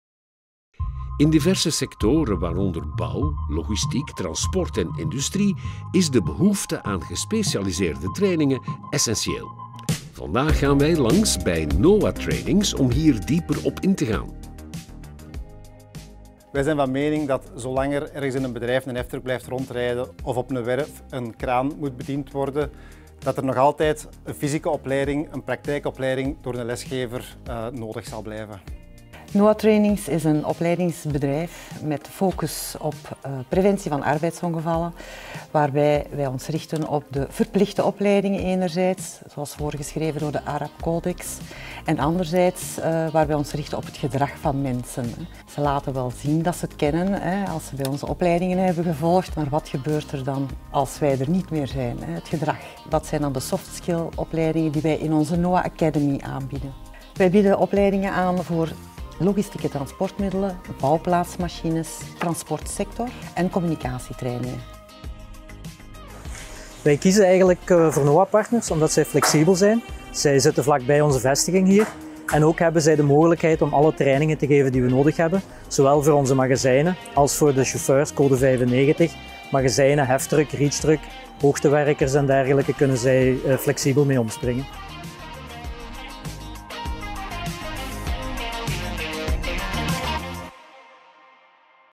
NOA Trainings
In diverse sectoren waaronder bouw, logistiek, transport en industrie is de behoefte aan gespecialiseerde trainingen essentieel. Vandaag gaan wij langs NOA Trainings om hier dieper op in te gaan.